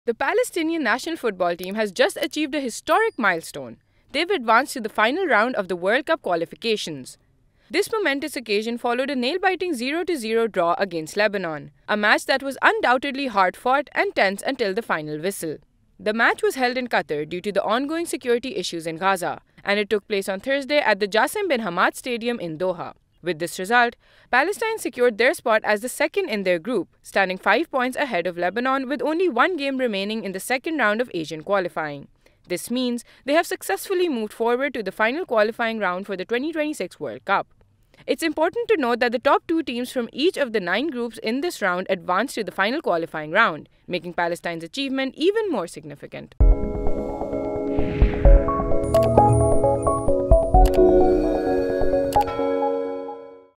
Palestine Advances to Final Round of 2026 World Cup Qualifiers After Lebanon Draw | Breaking News
The Palestinian national football team has reached a historic milestone by advancing to the final round of World Cup qualification. This incredible achievement followed a tense 0-0 draw against Lebanon at the Jassim Bin Hamad Stadium in Doha, Qatar, due to ongoing security issues in Gaza.

With this result, Palestine secured their spot as the second in their group, standing five points ahead of Lebanon with only one game remaining in the second round of Asian qualifying. This advancement to the final qualifying round for the 2026 World Cup marks a significant moment for Palestinian football.

The match was hard-fought and filled with tension until the final whistle, showcasing the determination and resilience of the Palestinian team. It’s important to note that the top two teams from each of the nine groups in this round advance to the final qualifying round, making Palestine's achievement even more noteworthy.

As Palestine moves forward, the nation celebrates this remarkable success, hoping for more triumphs on the global stage. This achievement not only brings pride but also strengthens the hope and unity of the Palestinian people.